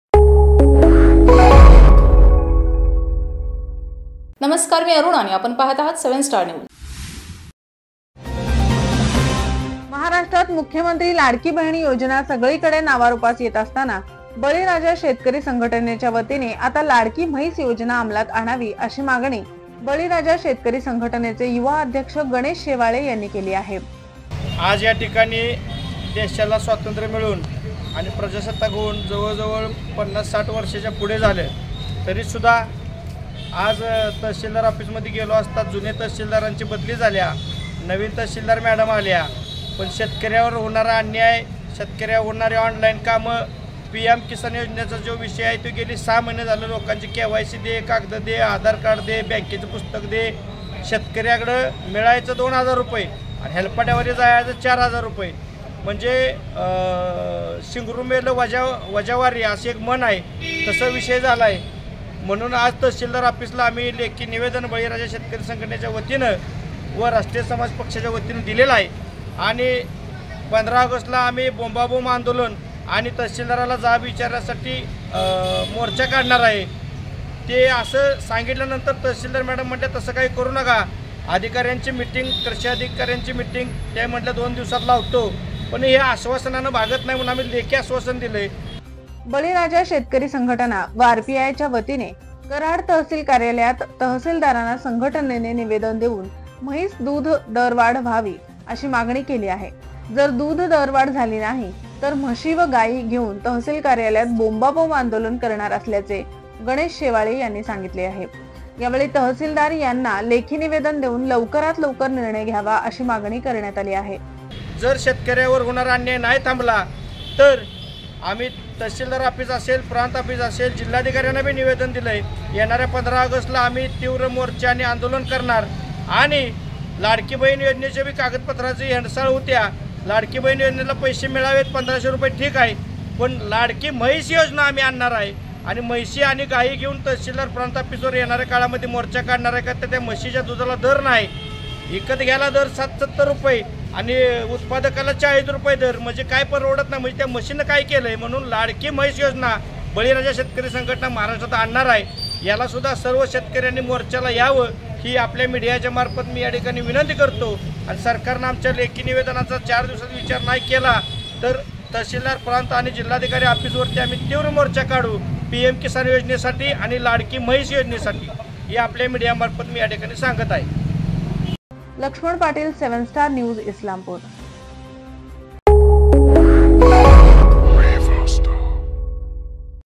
लाडकी बहीण नंतर आता येणार लाडकी म्हैस योजना तहसील कार्यालयसमोर बोंबाबोंब आंदोलन करण्याचा इशारा 7STAR
लाडकी बहीण नंतर आता येणार लाडकी म्हैस योजना
तहसील कार्यालयसमोर बोंबाबोंब आंदोलन करण्याचा इशारा